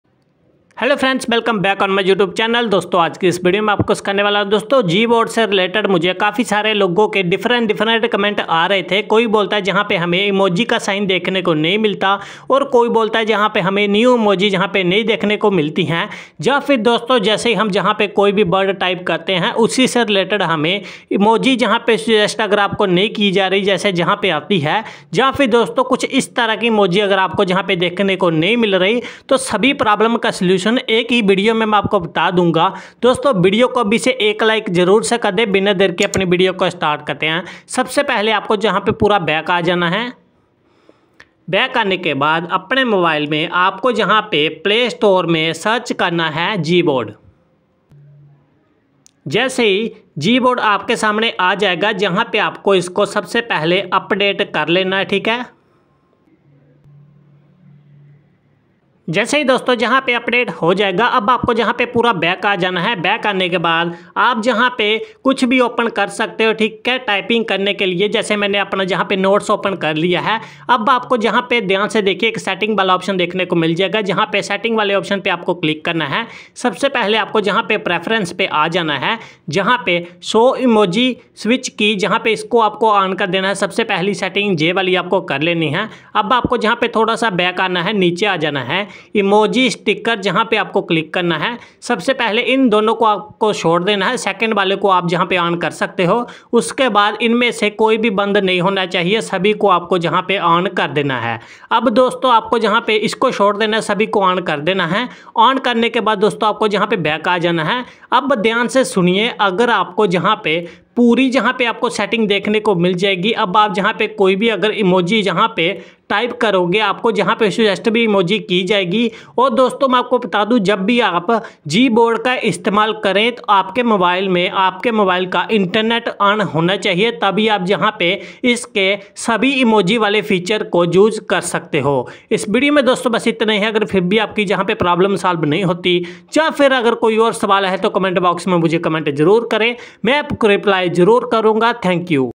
How to fix gboard new emoji not showing problem | gboard emoji stickers setting
Hello friends,
Is video me mai apko batane wala hoon ki agar apke phone me b gboard me new emoji and stickers show nahi ho rahe hai toh aap gboard me emoji aur stickers kaise la saktey hai.